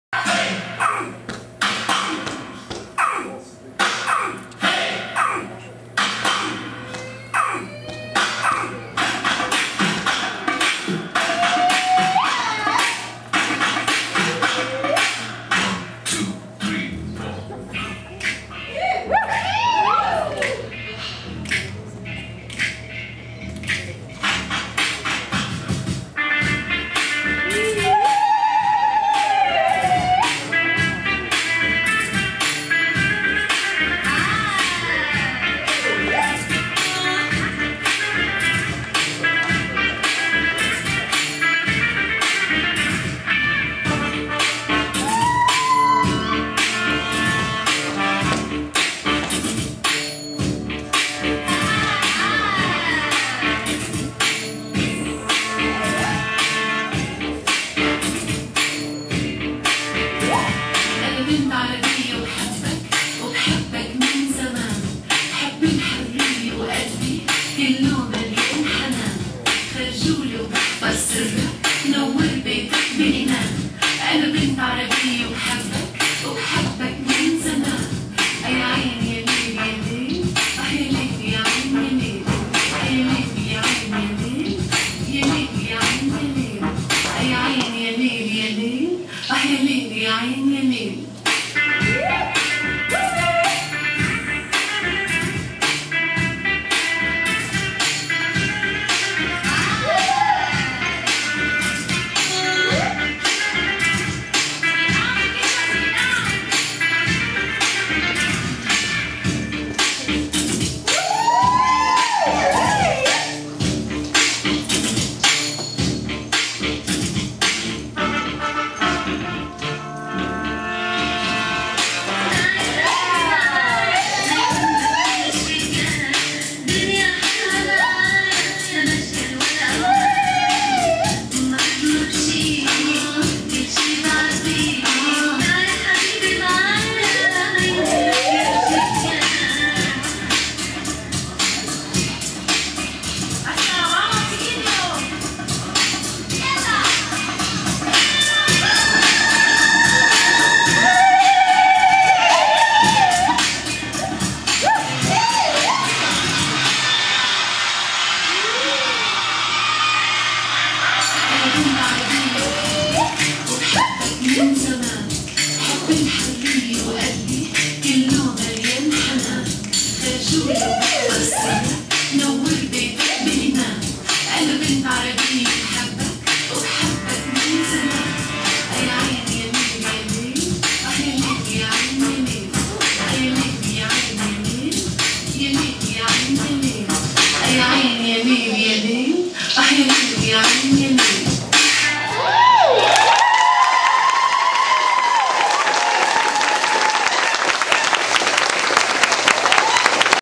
Marino Students 9-18-10
Jennifer Marino's CCAC students perform choreography at Bodiography Studios on September 18, 2010